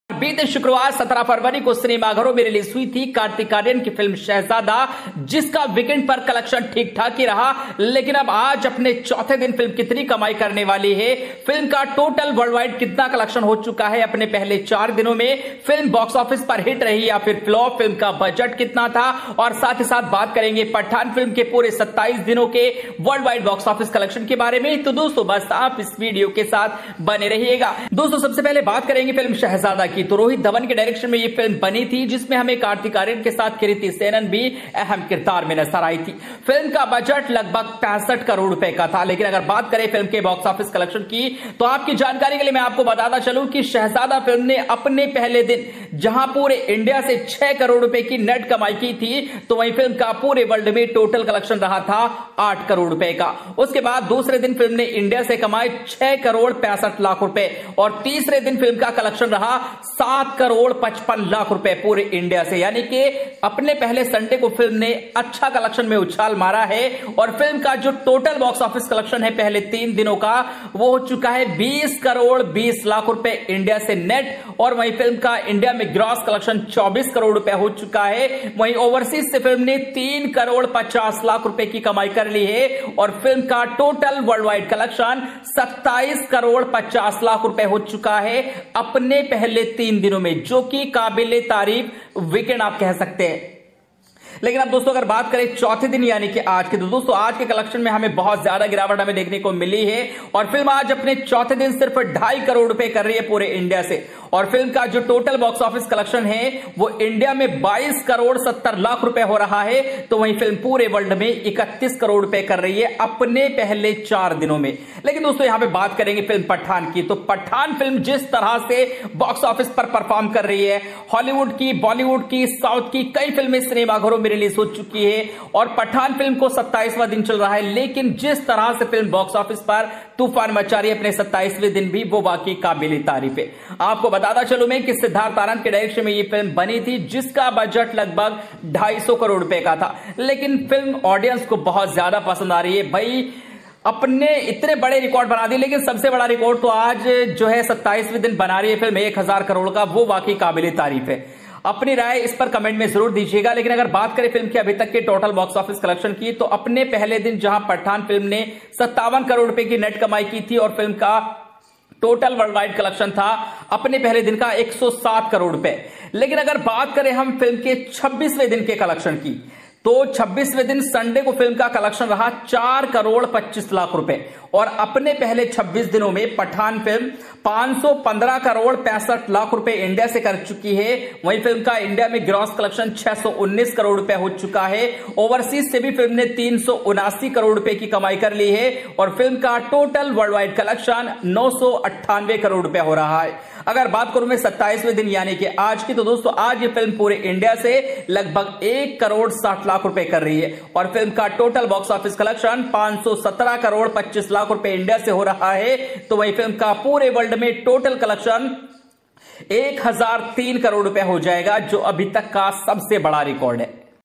Pathaan Vs Shehzada Box Office Collection। Shahrukh Khan, Kartik Aryan। Pathaan Collection।
About Pathan:–
Pathaan is an upcoming Indian Hindi-language spy action-thriller film written and directed by Siddharth Anand and produced by Aditya Chopra.[2] The film stars Shah Rukh Khan, Deepika Padukone and John Abraham.[2] It is the fourth installment in the YRF Spy Universe, after Ek Tha Tiger (2012), Tiger Zinda Hai (2017) and War (2019).